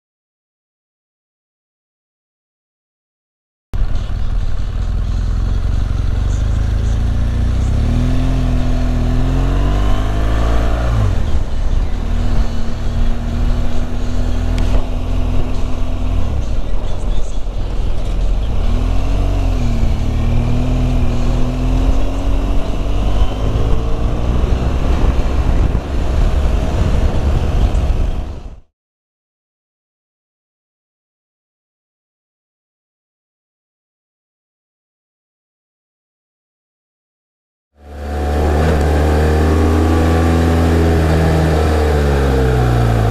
Texting and Driving, Chile, Jan 2020
bad driver (kid) moving a truck fall of propane tanks, while texting.